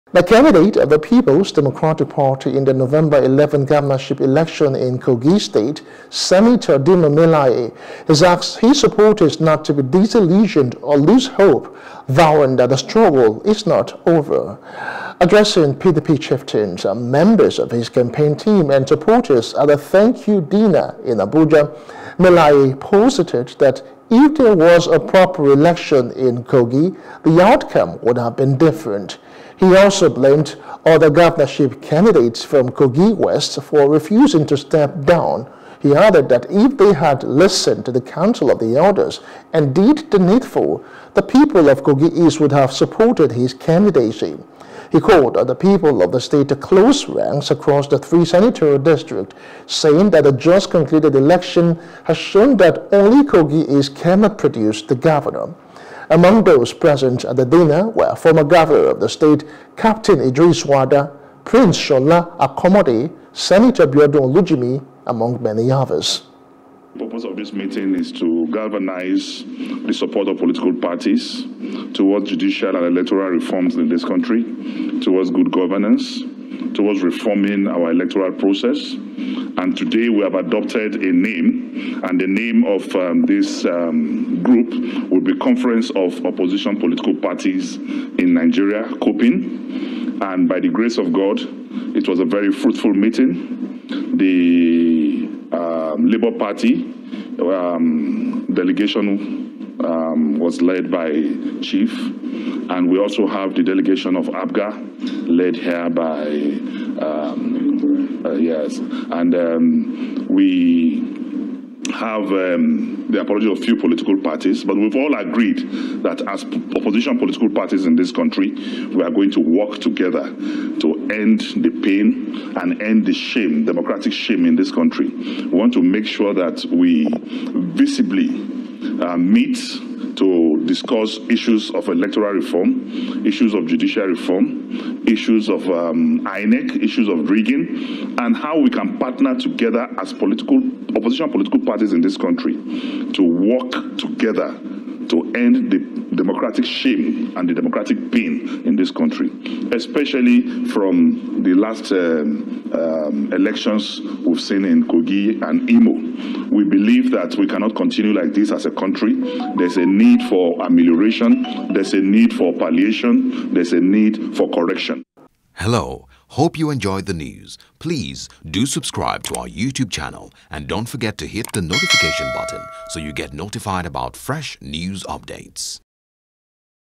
Kochi Governorship Poll: Melaye Vows To Bounce Back, Ask Supporters Not To Lose Hope
Candidate of the Peoples Democratic Party (PDP) in the November 11 governorship election in Kogi State, Senator Dino Melaye, has asked his supporters not to be disillusioned or lose hope, vowing that the struggle is not over yet.